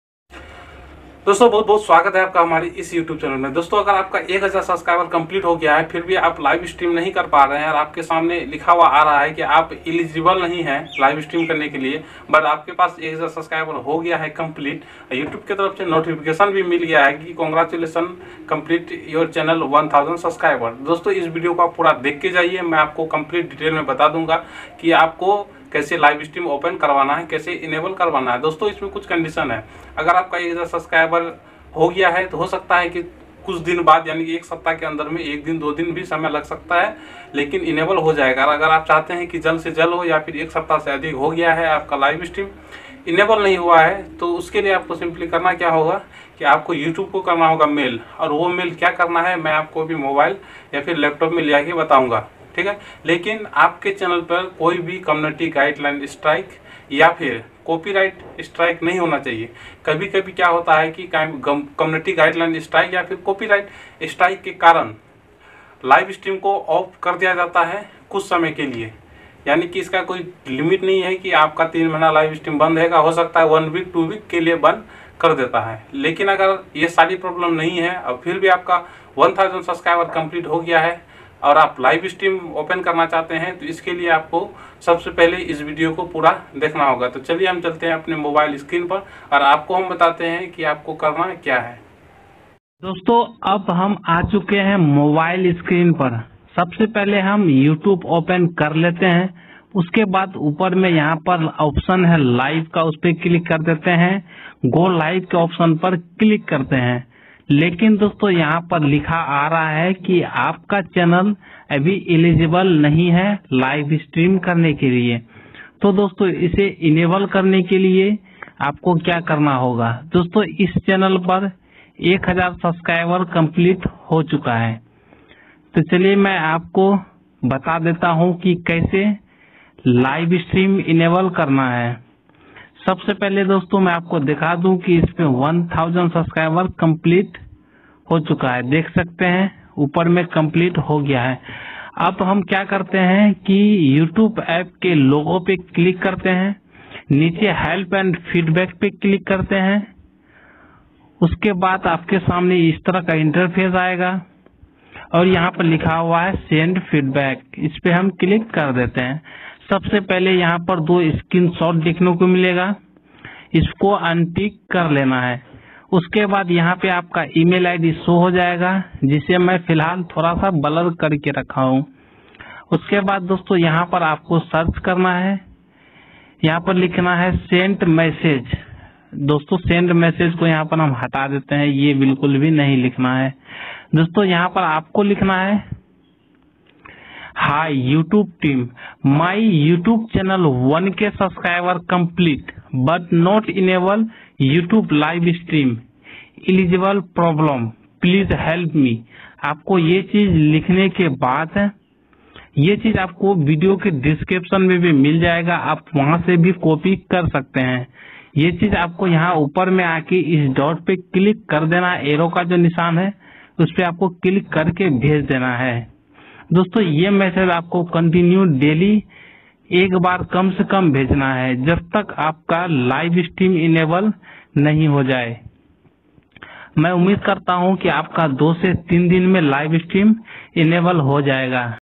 1K subscriber complete but not enable live stream | 2020
नमस्कार:-

send message

hi YouTube team

my YouTube channel 1K subscriber complete but not enable YouTube live stream
eligible problem